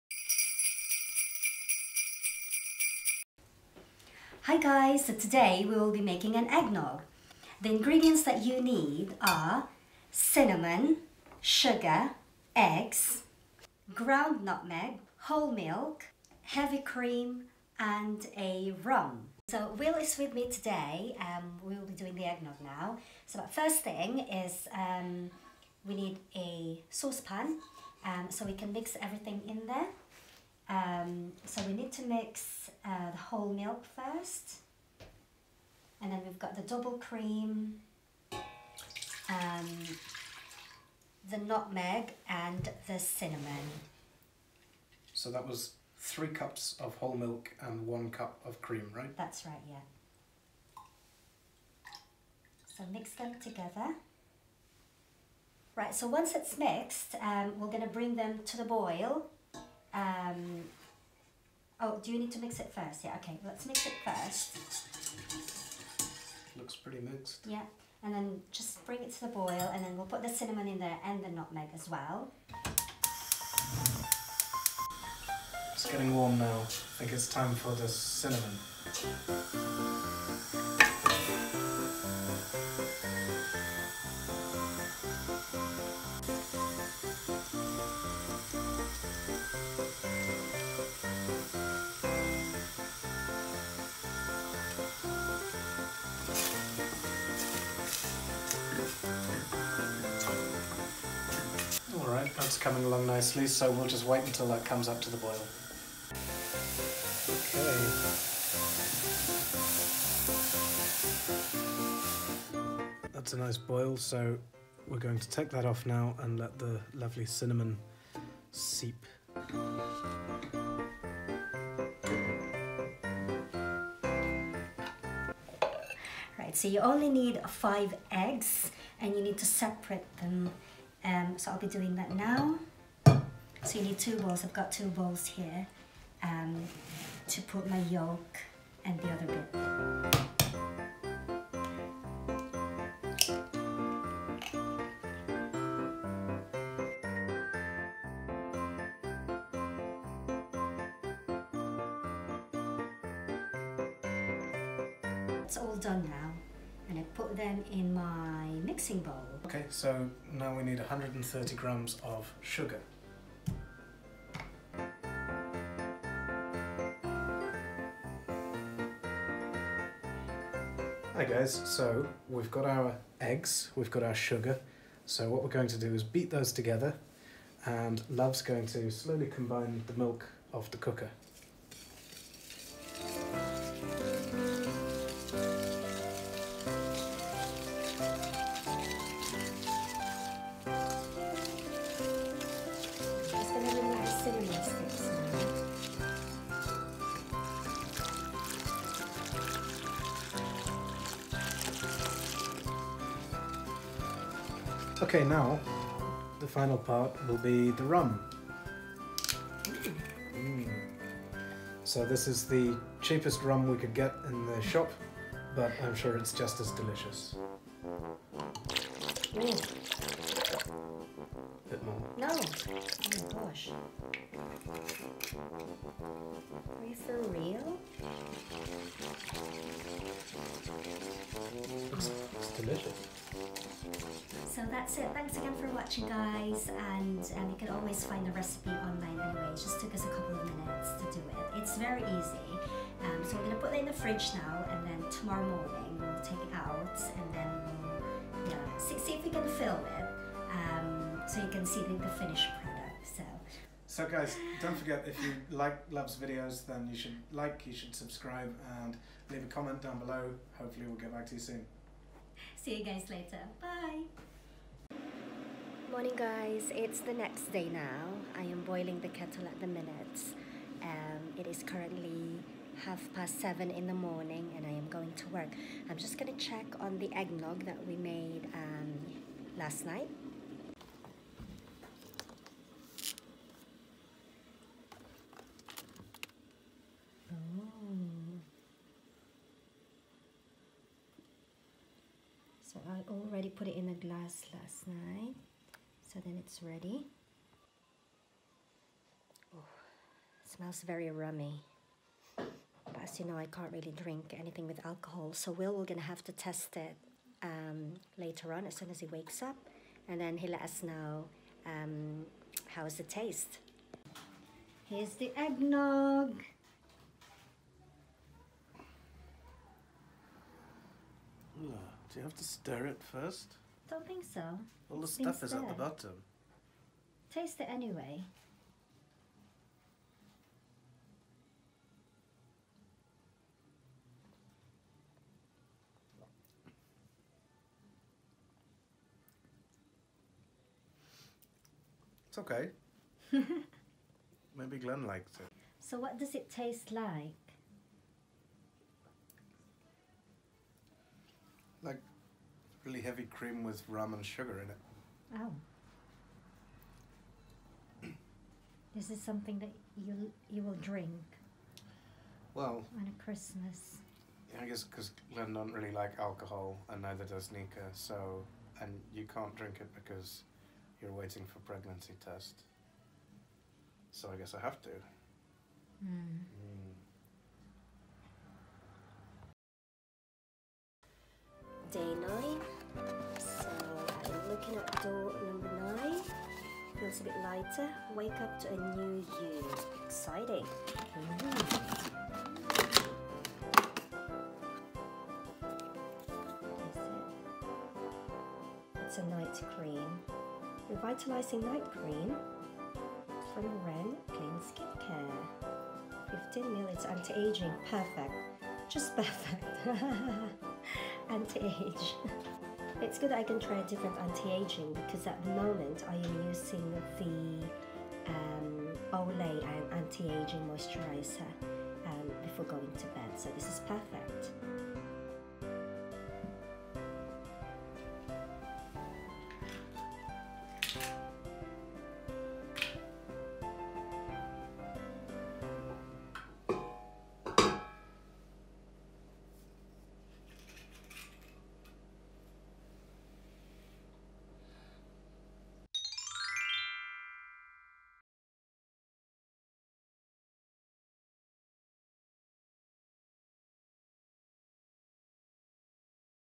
How to Make Eggnog | Very Easy - 5 Minutes | Vlogmas
Hi Guys - Today we are making egg nog!

You will need the following:
3 Cups milk
1 cup heavy cream
130gms sugar
Cinamon
Ground Nutmeg
5 eggs

It doesn't take very long at all. After we've made the Egg Nog we leave it to rest over night, I open my M&S Advent Calendar (day 9) and in the morning my husband drinks the egg nog.